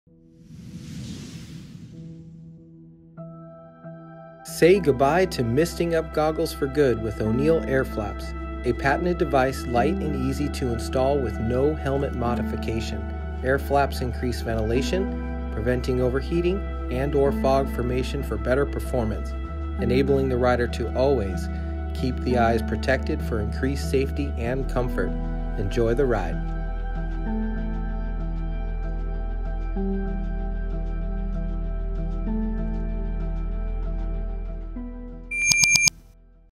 O'NEAL TECH ATTACK: AIRFLAPS™ KIT
Say goodbye to misting-up goggles for good with O'Neal Airflaps™. A patented device, light and easy to install with no helmet modification.  AIRFLAPS™ increases ventilation, preventing overheating and/ or fog formation for better performance, enabling the rider to always keep the eyes protected for increased safety and comfort.

The Airflaps system is an optional spacer that lifts the glasses off the face when needed, while still providing maximum eye protection. Ideal for hot conditions where increased air intake is of benefit.

Whether trail, enduro, downhill or motocross, with this system you always keep a cool head. The system is easy and quick to apply via adhesive tape - similar to a GoPro® mount - to most helmets, adjustable in relation to the distance to the face, preventing fogging of goggles and ensuring permanent air supply when in use. With the levers unfolded, there is no pressure on the nose and the helmet can also be put on and taken off without removing the goggles.

The O'NEAL Airflaps Kit is available for 29.99€ and has already proven itself in use on track with several top riders, including the 9-time MX World Champion Tony Cairoli.

THE ADVANTAGES OF O'NEAL AIRFLAPS:

· AIRFLAPS increases ventilation inside the google and under the helmet
· Works by lifting the goggle foam a few mm offw
· Also allows the helmet to be put on and taken off without removing the goggles and reducing scratching
· Prevents overheating for a better performance
· Compatible with Roll-Offs and Tear-Offs
· Works by lifting the goggle foam a few mm off the face
· Single hand operation while riding
· The distance between the goggle foam and the face can be adjusted easily by up to 10mm thanks to a single adjustment screw
· Simple installation with 3M double face fixation, no tools required